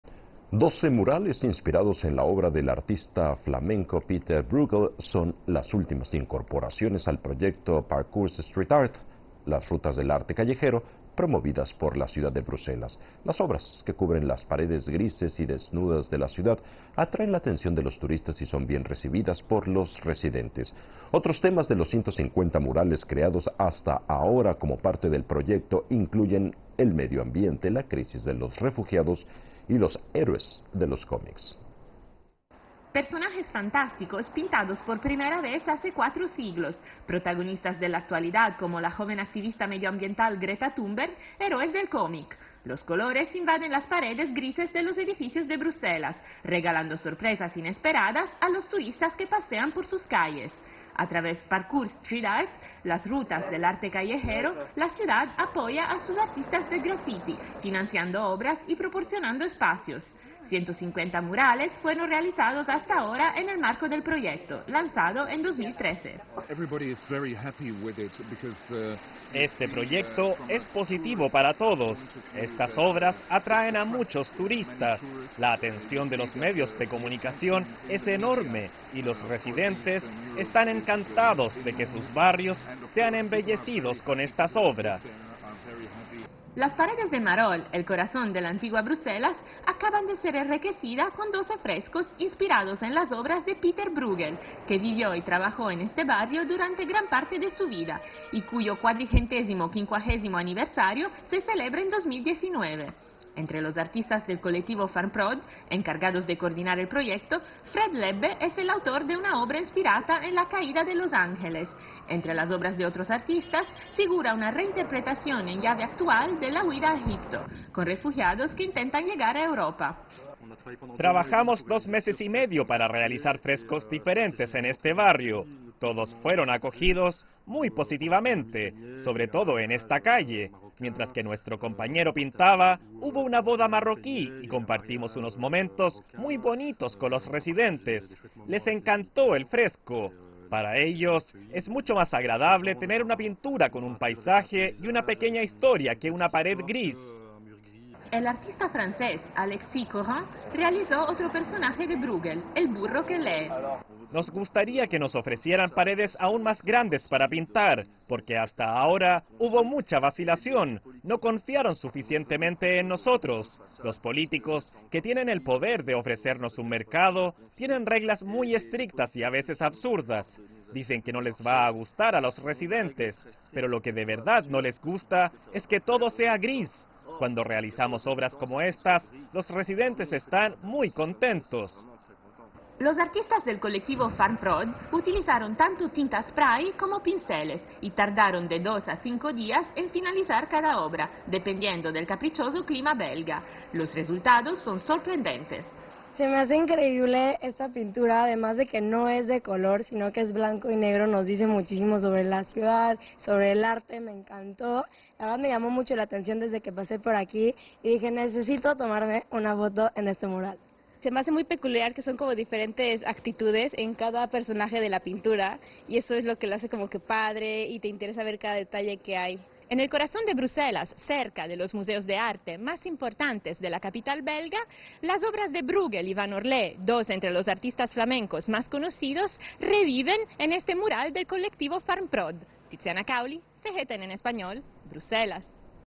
Las obras Pieter Bruegel y personajes de la actualidad inspiran a los autores de graffitis
Doce murales inspirados en la obra del artista flamenco Pieter Bruegel son las últimas incorporaciones al proyecto "Parcours Street Art", las "rutas del arte callejero" promovidas por la ciudad de Bruselas. Las obras, que cubren las paredes grises y desnudas de la ciudad, atraen la atención de los turistas y son bien recibidas por los residentes. Otros temas de los 150 murales creados hasta ahora como parte del proyecto incluyen el medio ambiente, la crisis de los refugiados y los héroes de los cómics.